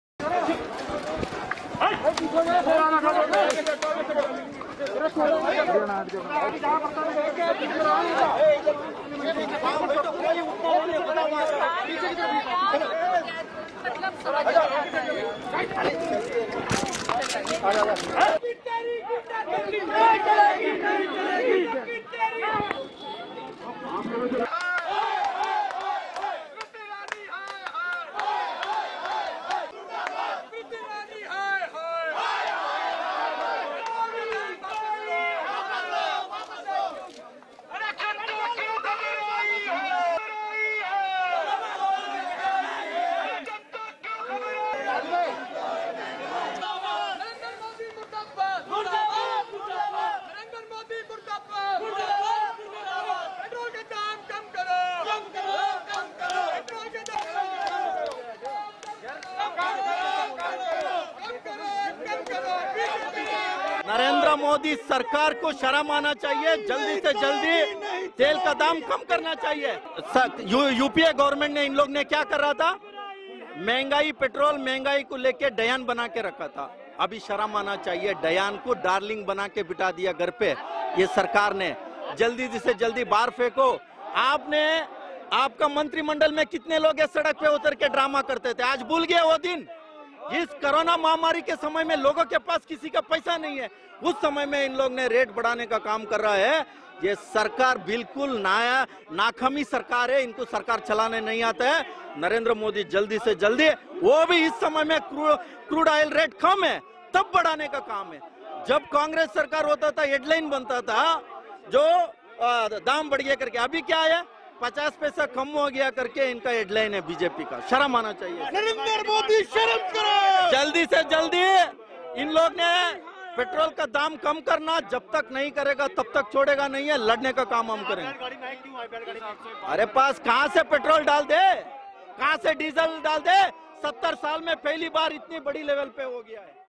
National President of Indian Youth Congress Shri srinivas bv. Protest Against Fuel Price Hike
National President of Indian Youth Congress
Shri srinivas bv. Protest Against Fuel Price Hike